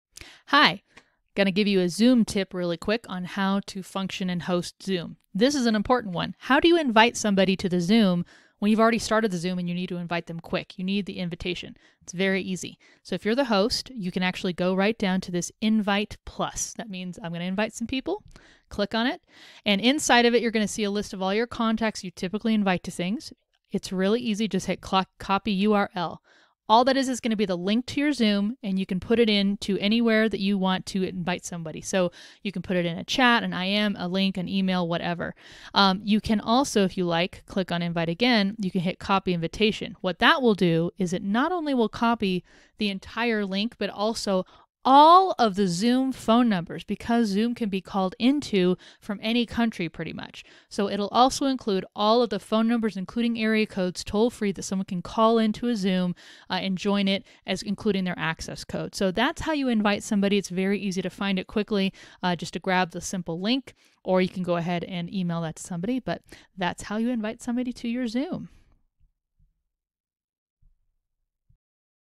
How Can I Invite People to My Zoom Meeting | Really Social
If you're already in a Zoom and need to invite someone to it, this video will show you the quick ways you can send the link or the full invitation to someone easily.